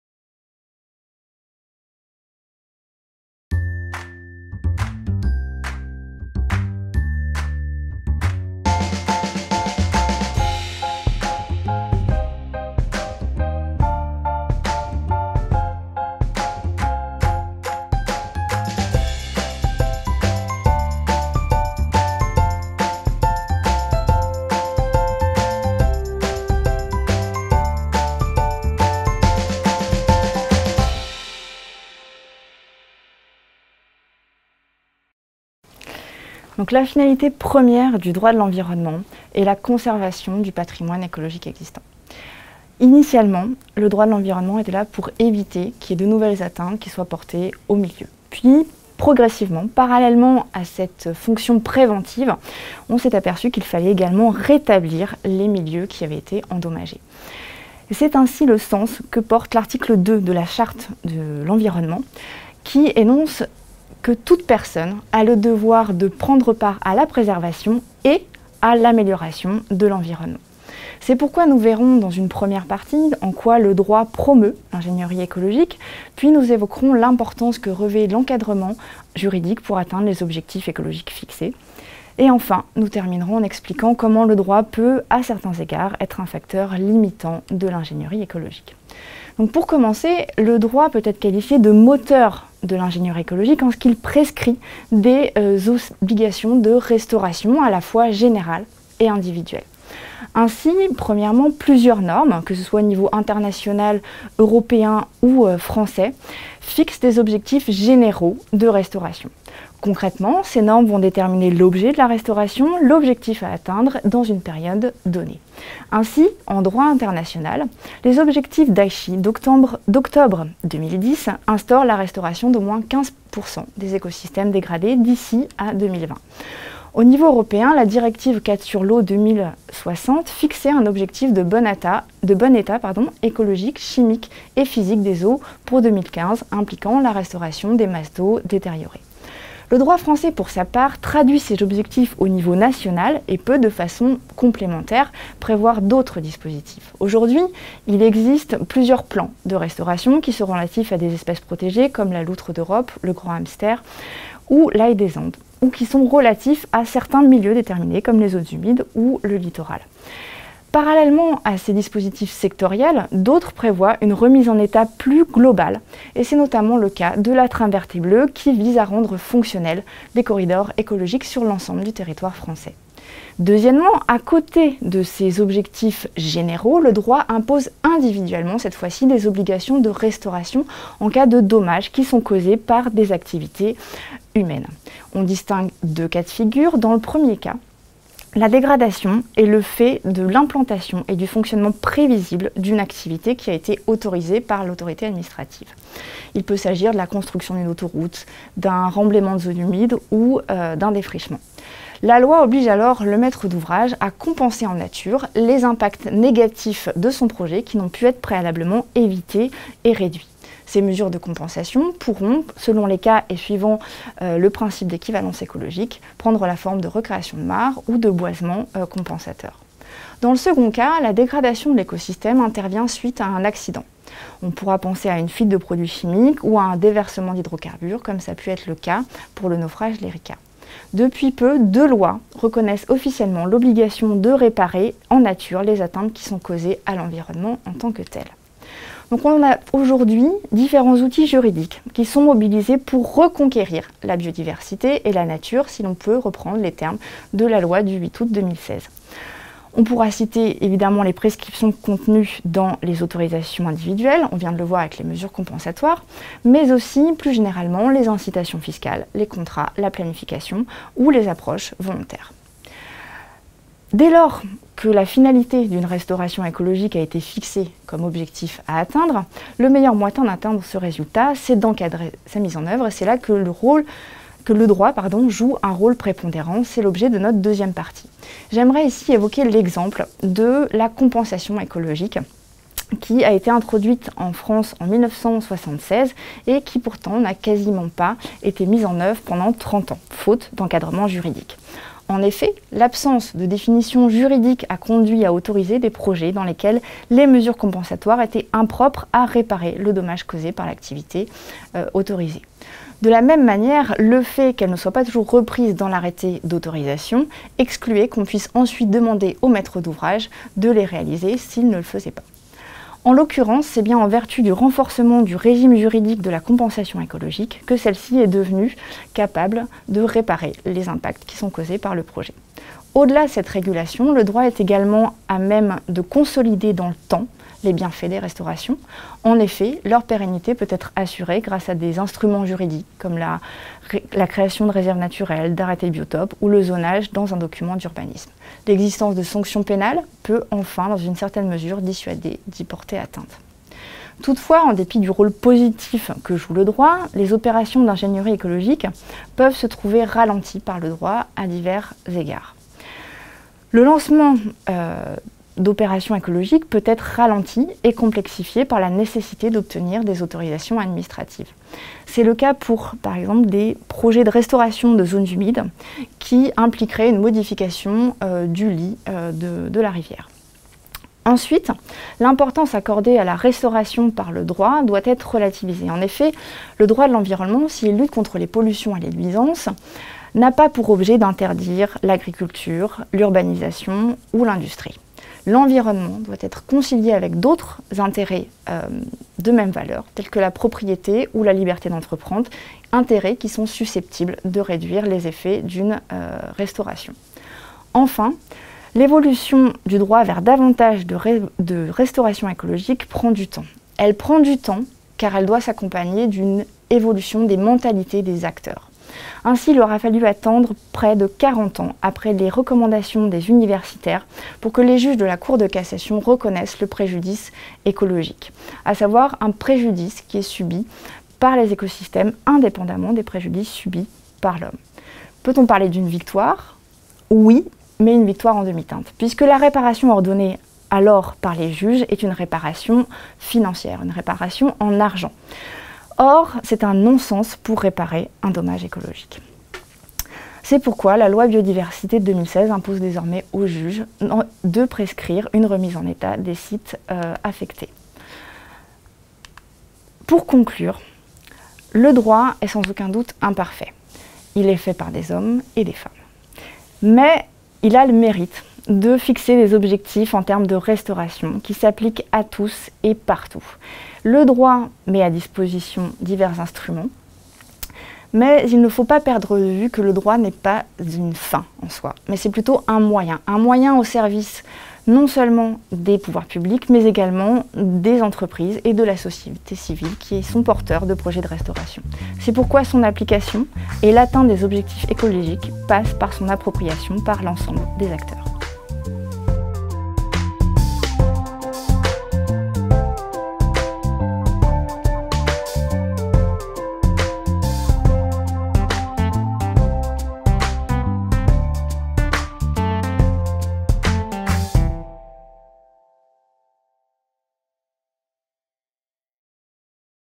Le droit et l'ingénierie écologique : une approche par le prisme de la restauration écologique
Marthe Lucas, maître de conférence à l'Université d'Avignon, examine dans cette vidéo (8'26) la double contribution du droit dans le déploiement des pratiques d'ingénierie écologique. Elle montre d'une part que le droit est porteur d'obligations pour restaurer des écosystèmes ou pour compenser des impacts écologiques, et d'autre part que le droit est utile pour encadrer la mise en œuvre de ces pratiques. Elle conclut par une discussion sur les intérêts et les limites des instruments juridiques dans la perspective d'une préservation et d'une réparation des écosystèmes.